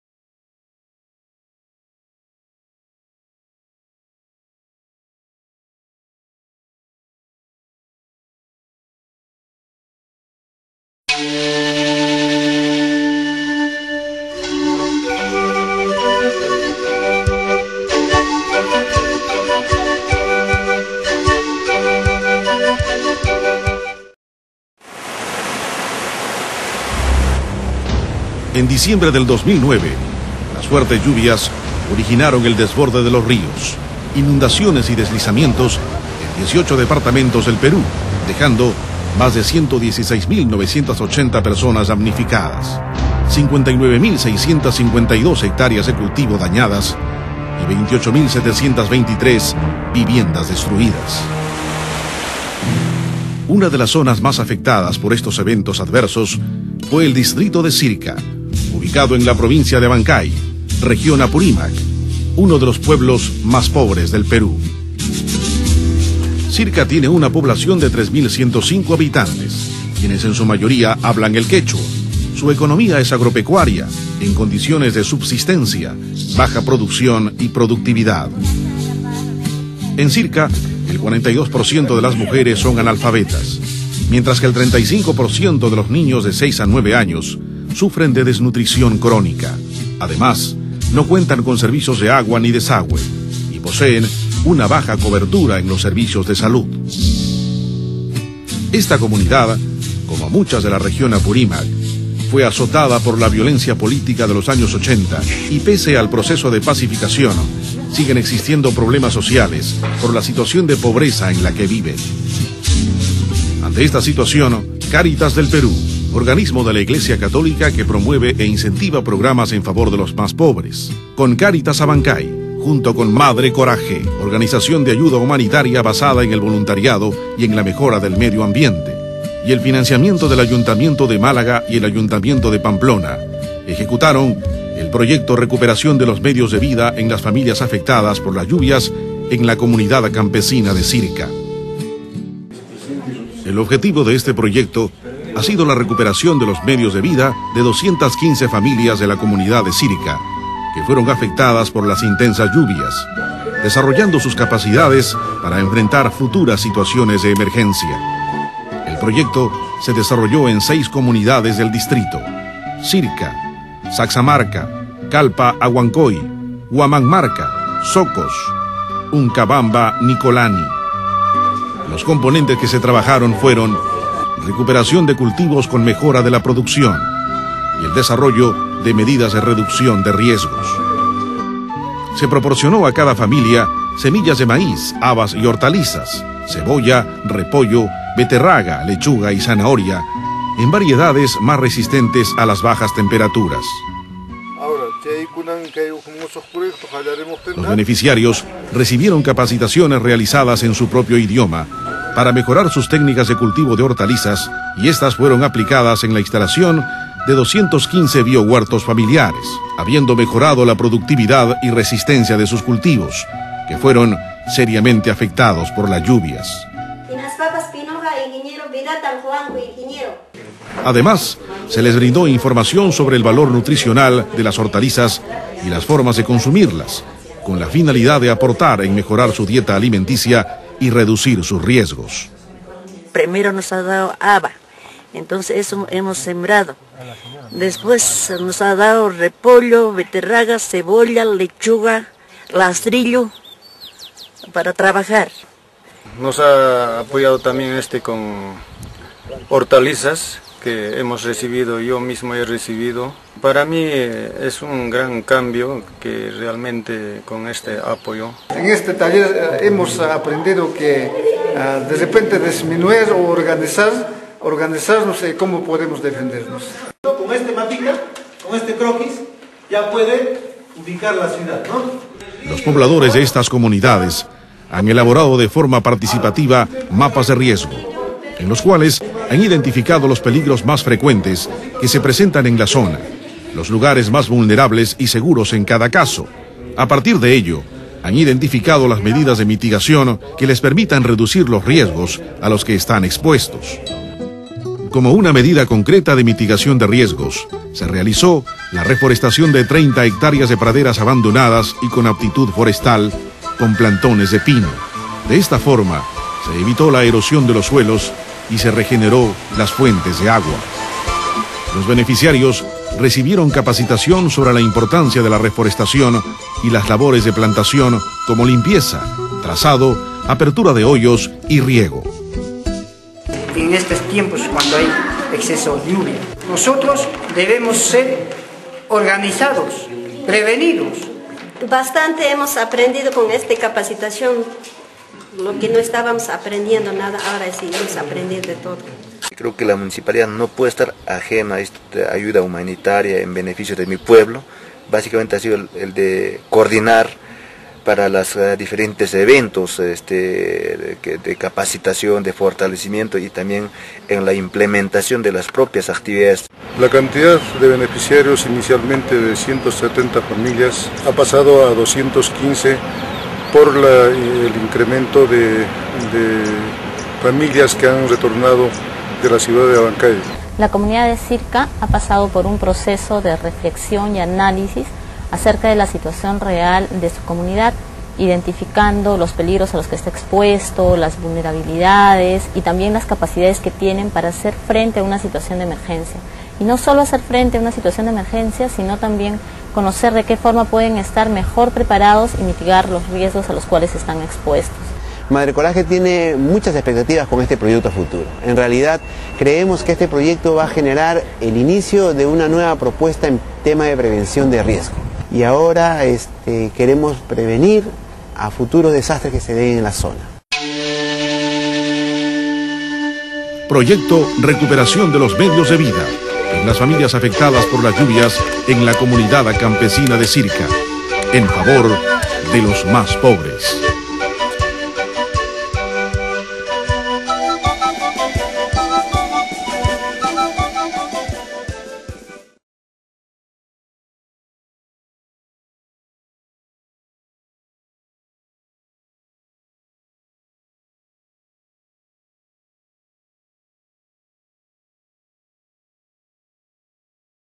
Apoyo a la comunidad de Circa (Apurímac-Perú)
La ONG para el Desarrollo Madre Coraje, con la financiación del Ayuntamiento de Málaga y del Ayuntamiento de Pamplona, ha concluido el proyecto "Recuperación de los medios de vida en las familias afectadas por las lluvias en la comunidad de Circa", puesto en marcha por Cáritas del Perú con la participación de los pobladores de 6 comunidades del distrito peruano de Circa.